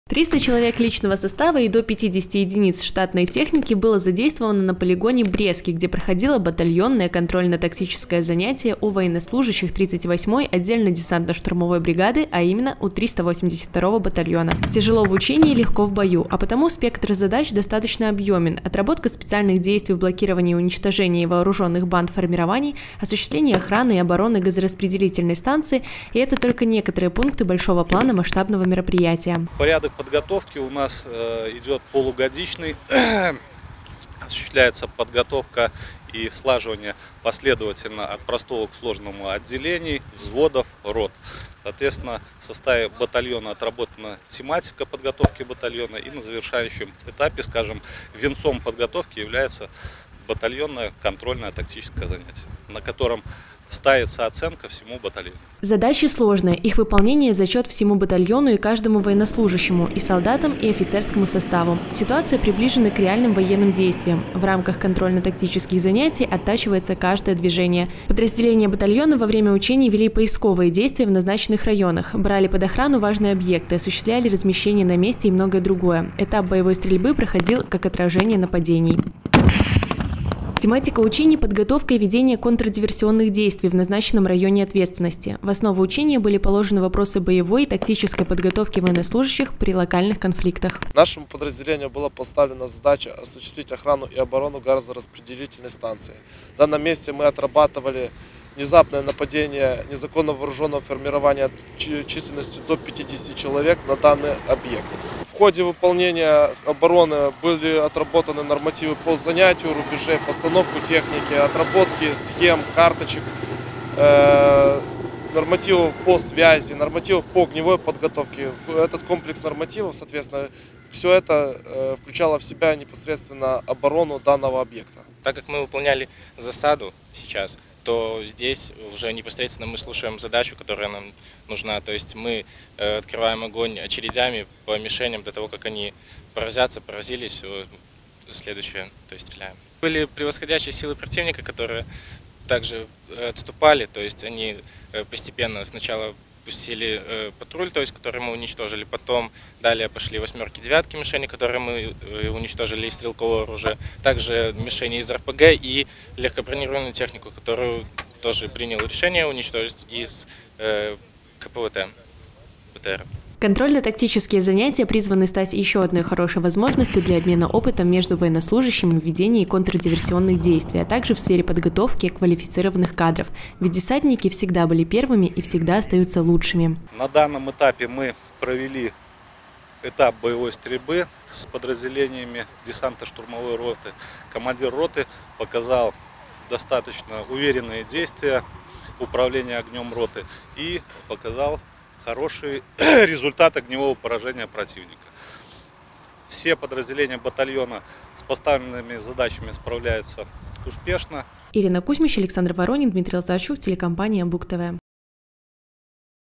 2020-04-08 г. Брест. Контрольно-тактические занятия в 38-й ОДШБ. Новости на Буг-ТВ.
Военнослужащие 38-й отдельной гвардейской Венской Краснознамённой десантно-штурмовой бригады вновь на учебной передовой. Во время проведения контрольно-тактических занятий они показывают   навыки боевой подготовки:   десантники отрабатывали специальные действия в блокировании и уничтожении вооруженных банд формирований, осуществляли охрану и оборону важных объектов, а также выполняли другие задачи. Как проходит завершающая фаза батальонных тактических учений, узнавала Ирина Кузьмич.